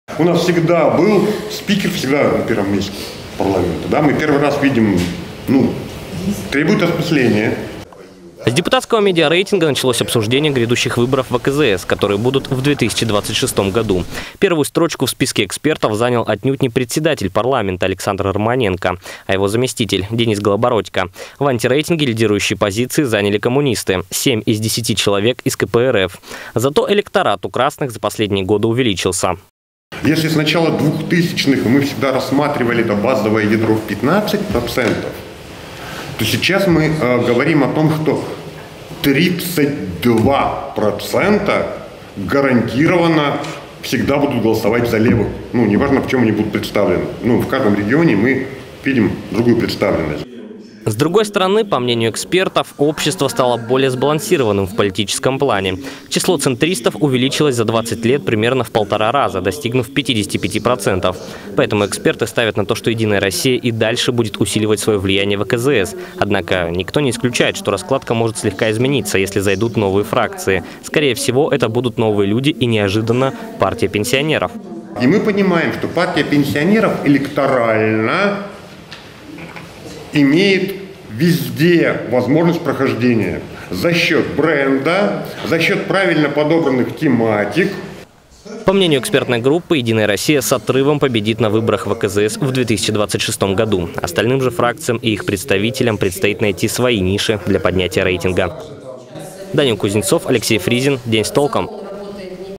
Эксперты обсудили грядущие выборы депутатов АКЗС
В 2026 году в АКЗС могут появиться представители двух новых партий. Таким мнением поделились политические эксперты Алтайского края во время круглого стола касательно грядущих выборов членов заксобрания. Также политтехнологи озвучили медиарейтинг депутатов. Какое политическое настроение сейчас превалирует у избирателей и какая фракция выглядит наиболее перспективной – смотрите в сюжете Данила Кузнецова.